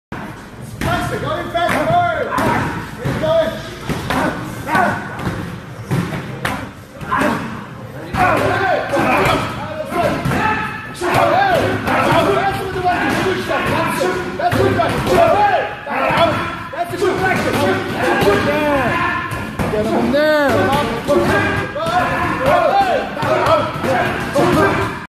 Erick Silva and Stephan Pütz
UFC's Erick Silva during a session with former M1 Light heavyweight champion Stephan Pütz